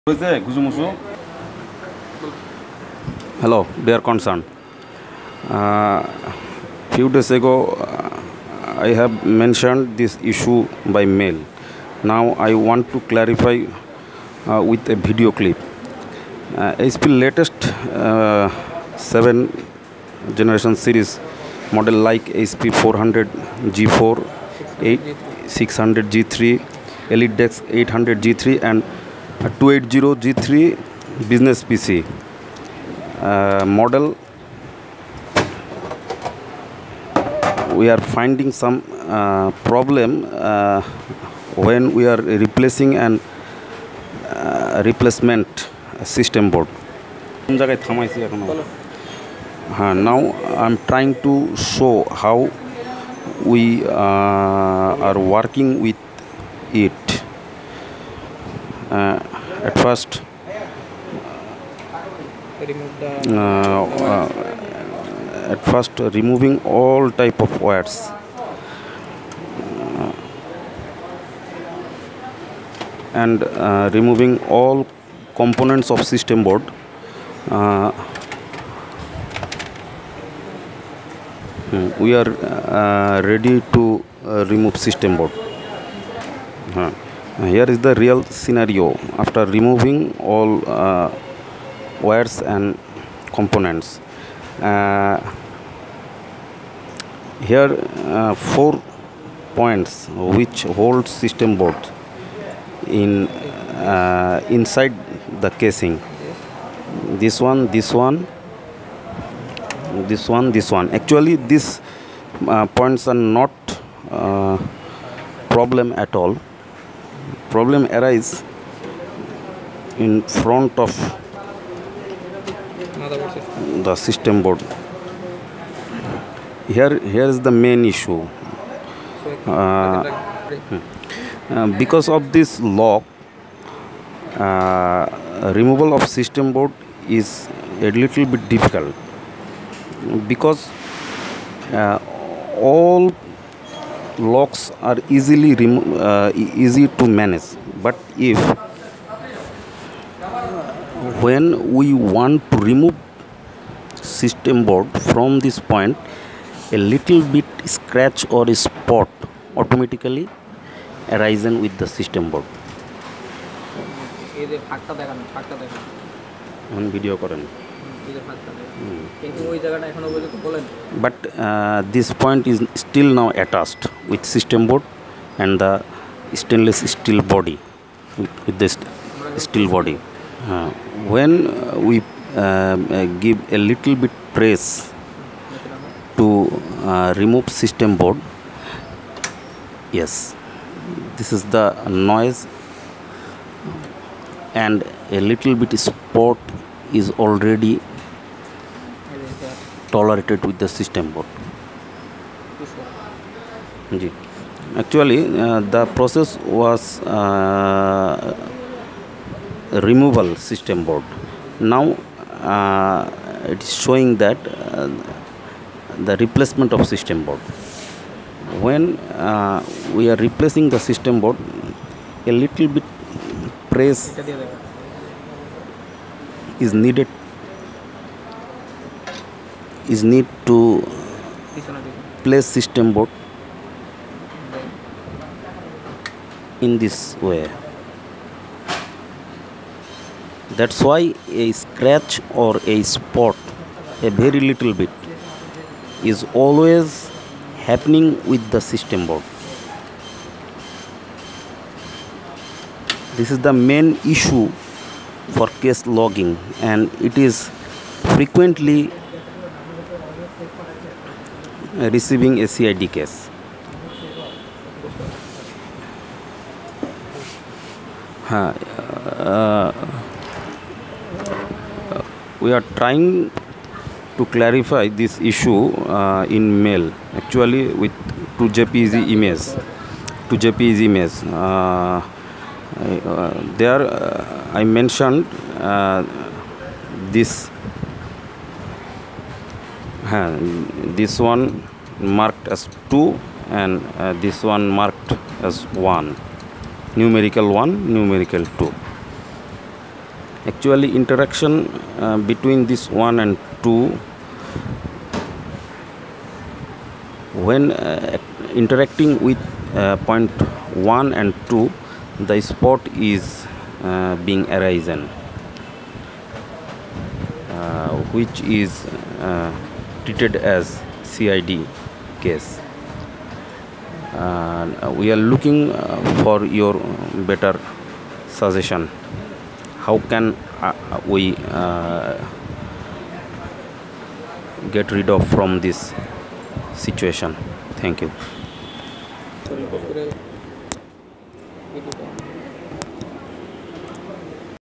How To Disassemble hardware from HP Prodesk 400 G4 MT Desktop Computer
Hi
Dear Tech Tunes. Today here we come with a new Video About Basic Computer hardware introduction.